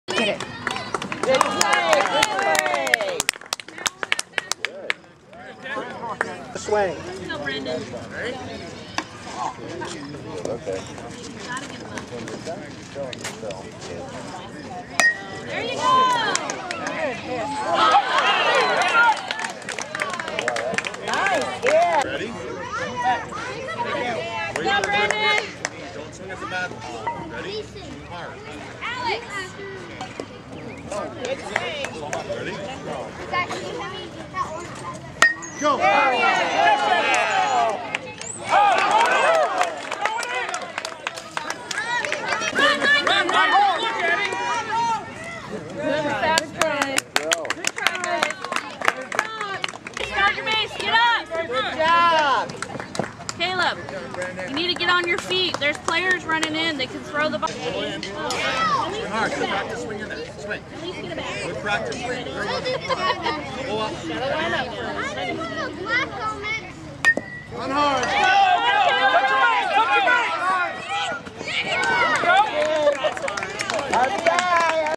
Baseball Game - May 25, 2016 (3rd Grade)
Playing 1st base and hitting